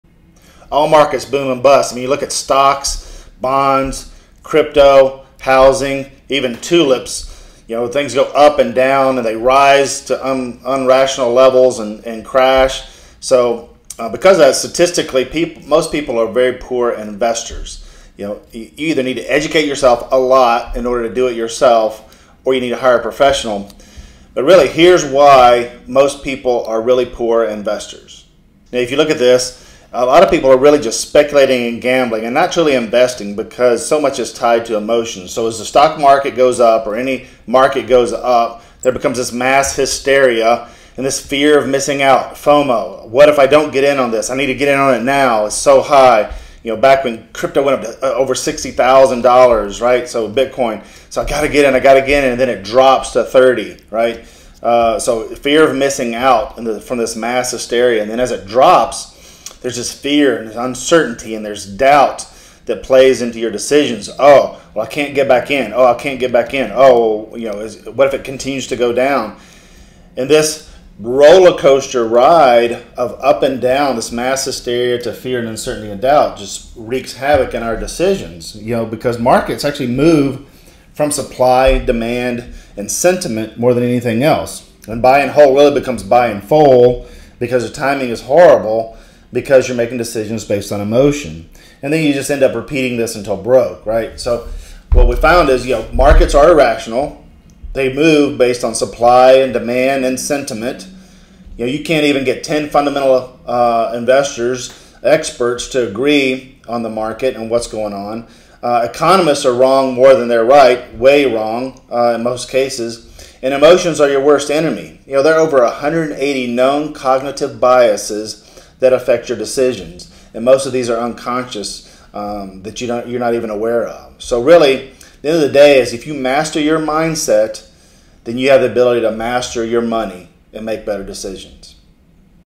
Boom or Bust
All markets boom and bust!

From stocks to bonds to crypto to housing to tulips.

Statistically, most people are poor investors.

The primary reason is emotion. We make emotional decisions when investing based on the rise and fall (booms and busts) of the market.

There’s mass hysteria when markets rise and fear/doubt when markets fall. It has been said that the stock market is the only “store” where we buy when things are expensive and sell when things go on sale.

Emotions are your worst enemy.

Master your mindset and you will master your money!